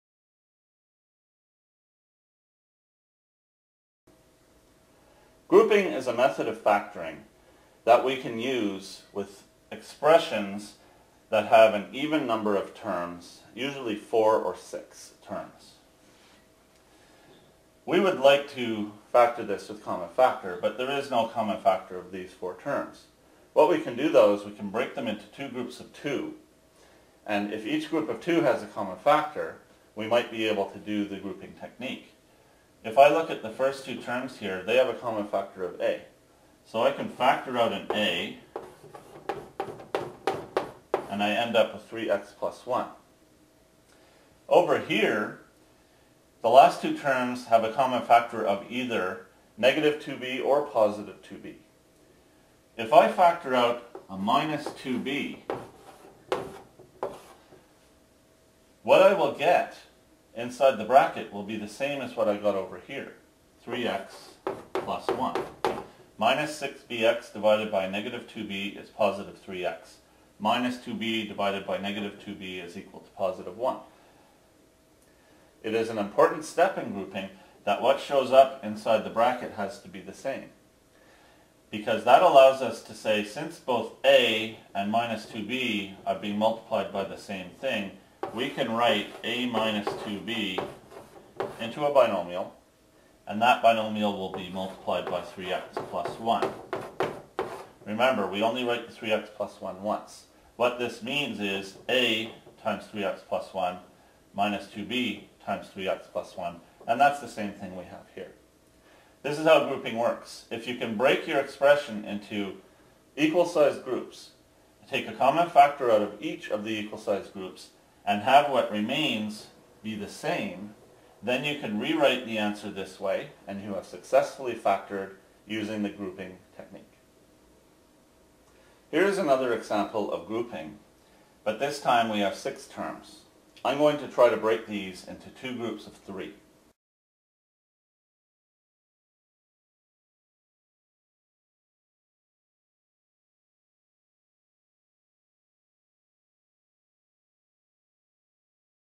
Factoring by Grouping
This video illustrates how to use the Grouping method to factor a polynomial expression. For part 2 of this video, including examples 2 and 3, as well as many more instructional Math videos and exercise and answer sheets, go to: